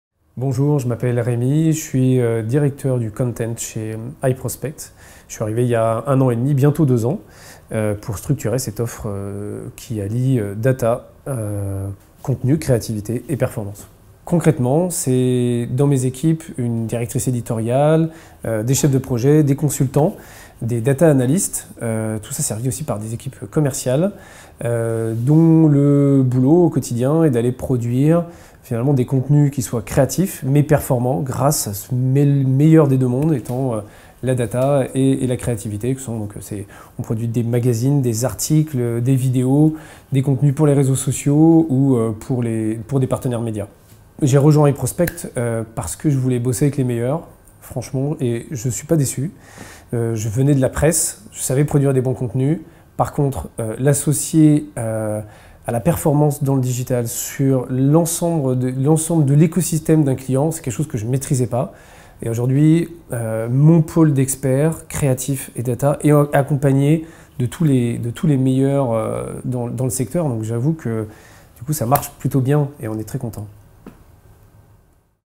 Découvrez iProspect avec Rémy, Directeur Contenu, Marketing & Communication
iProspect recrute !